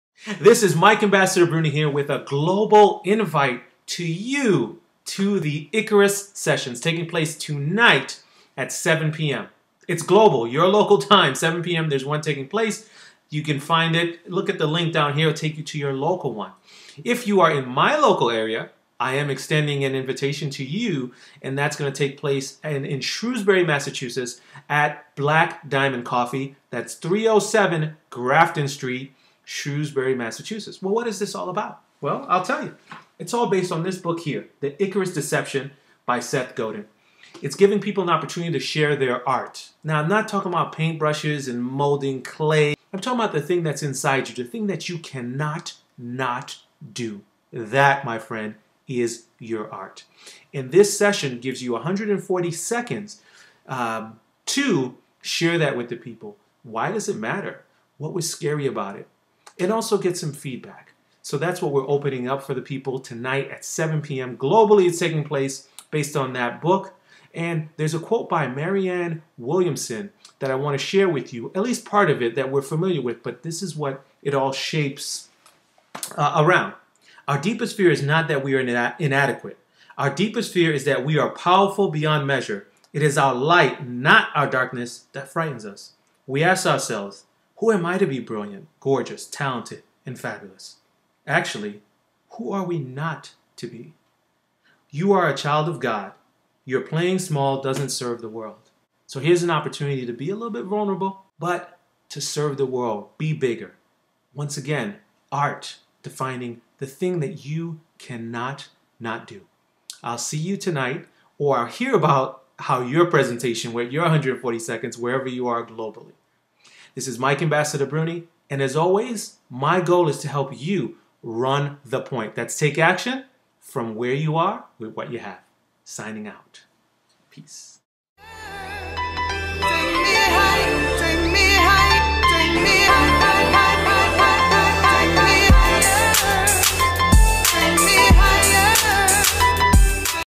Icarus Sessions In Shrewsbury and Globally
This video focuses on the Icarus Session which is based on Seth Godin's book, "The Icarus Deception."  It's all about allowing people the space to share their art in 140 seconds.  Art is defined as the thing you cannot, not do (not the traditional sense of the word).

It's taking place on Wednesday, January 2nd at 7 p.m. (Your local time).  I will be holding a local one in Shrewsbury, MA with a couple of friends.

In the area?  We'd love to have you.  It's at "Black Diamond Coffee; 307 Grafton Street, Shrewsbury, MA"

Here's to doing what we cannot, not do.

As always my goal is to help you Run the Point (Take Action) from where you are, with what you have.

Your Ambassador,
Mike Bruny